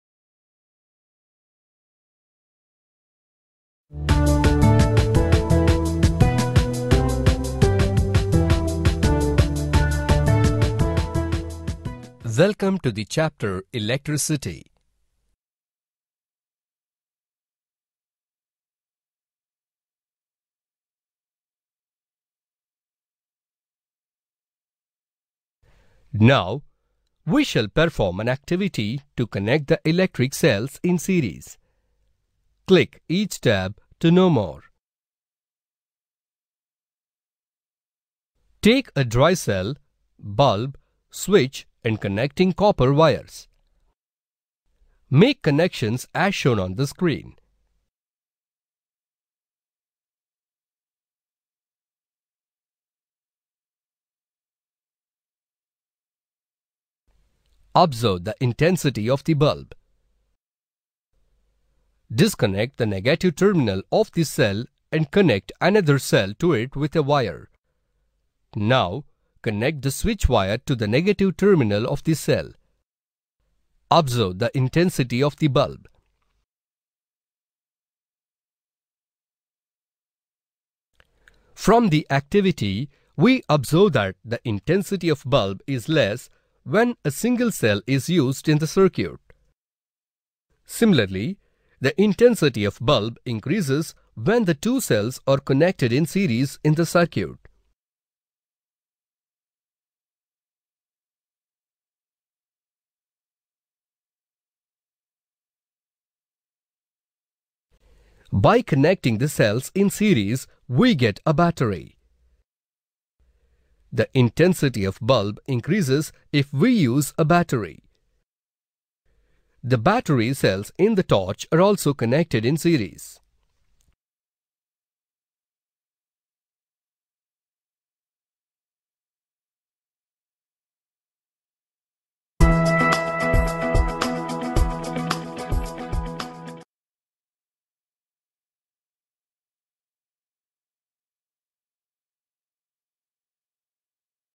Connecting Electric Cells in Series - Electricity, Class 7 Physics | SSC
Electricity - Now, we shall perform an activity to connect the electric cells in series
Digital Teacher is an extraordinary blend of technology and innovation that helps learners to retain the knowledge permanently!